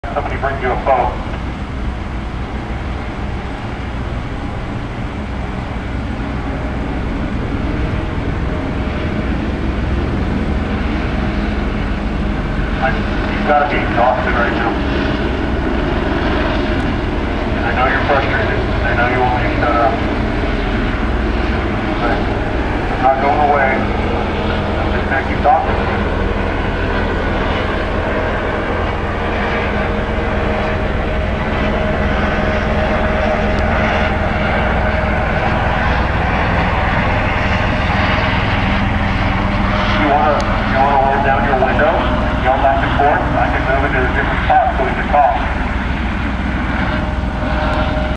Nevada County Homicide Suspect I-80 at Longview Dr. 3-21-2016 (Video by Tracy Mapes©2016
Police and Sheriff's from Nevada, Placer and Sacramento County draw weapons while they wait out a fleeing murder suspect that led them on a chase from Nevada County to I-80 and Longview Dr. in Sacramento County. The standoff lasted for two hours, and left motorists to seek detour routes because the freeway was shutdown in both directions. March 20-21, 2016. (Not For Broadcast). (Video by Tracy Mapes ©2016).